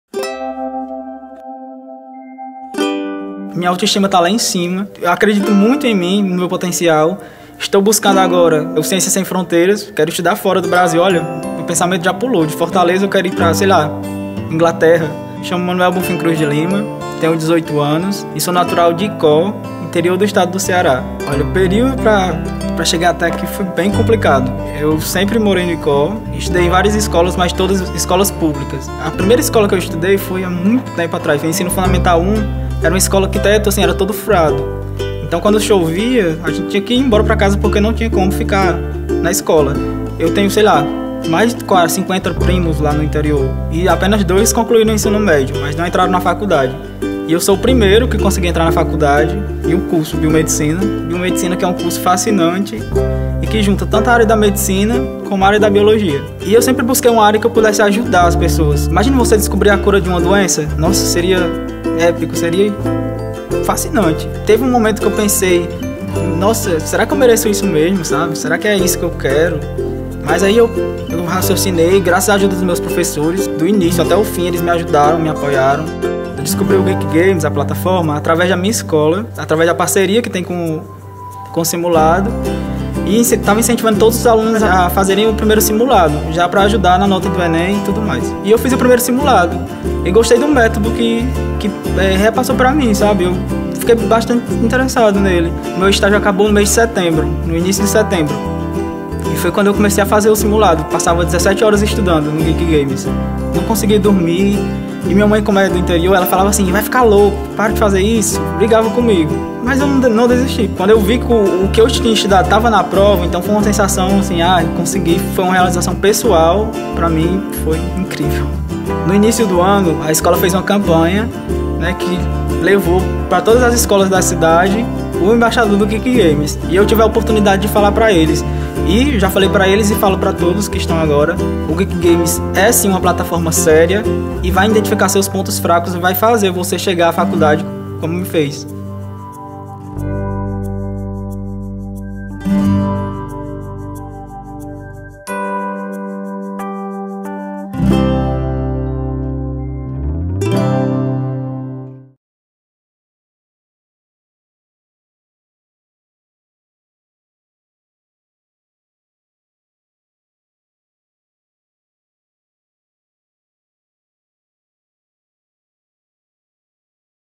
Geekie Games - A história de sucesso do Manoel
O Manoel se deu bem no ENEM! Conheça a história dele e como o Geekie Games o ajudou! Para saber mais, acesse